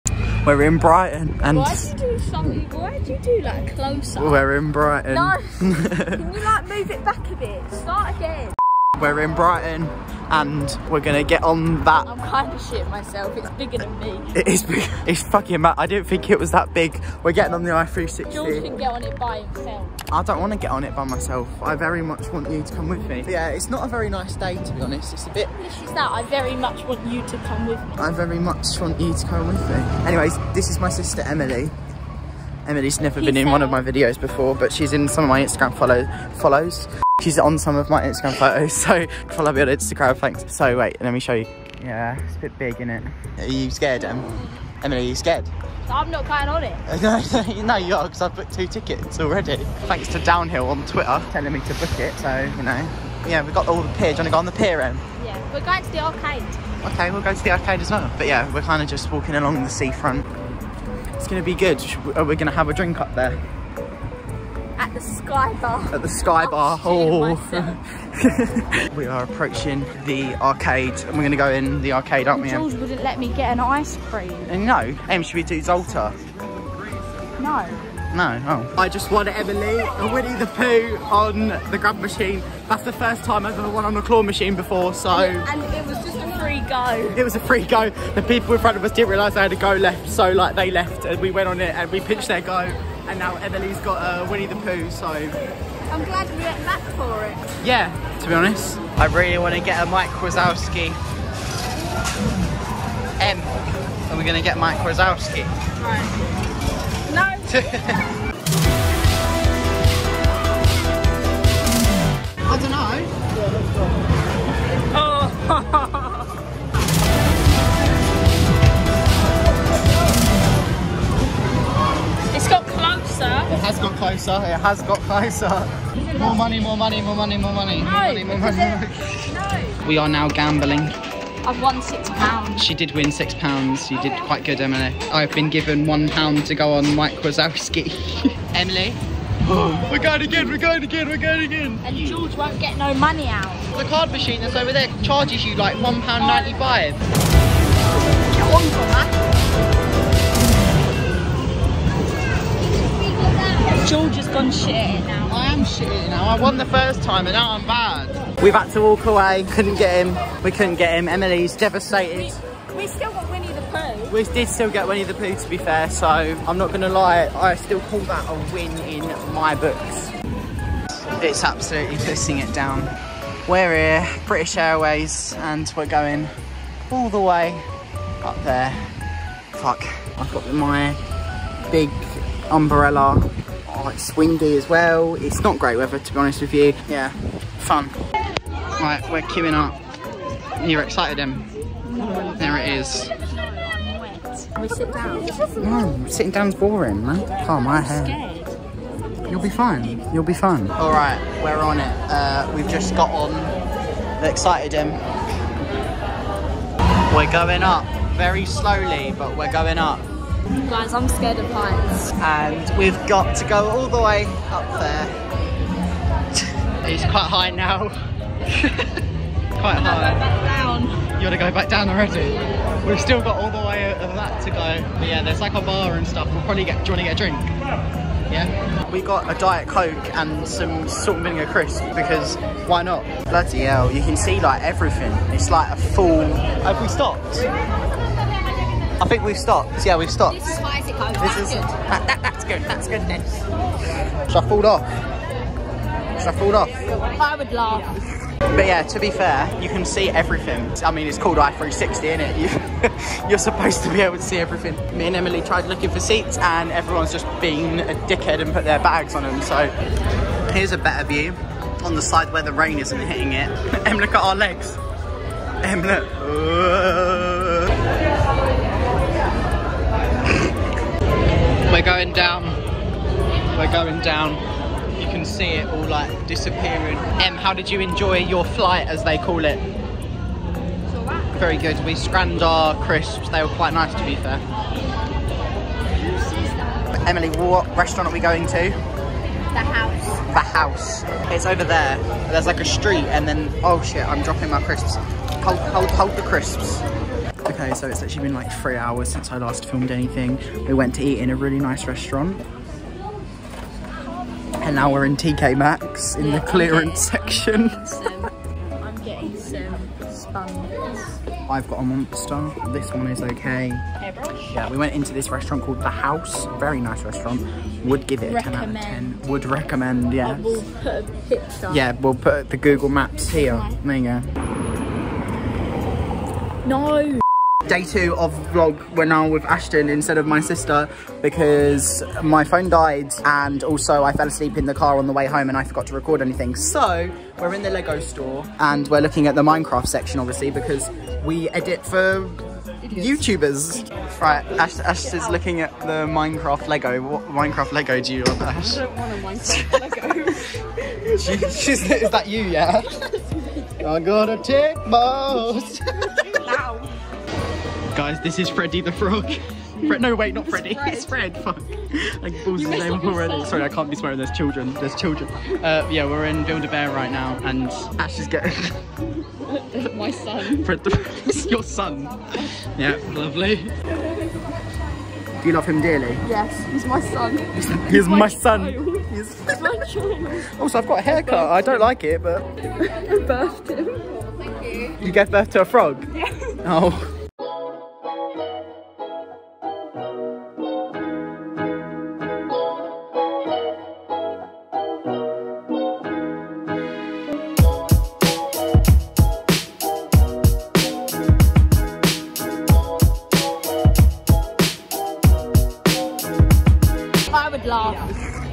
Brighton's i360 is SCARY... | georgey buckley
Brighton's i360 is SCARY...

YO! in this video i went to brighton's biggest landmark, the i360! also little section at the end with ashton x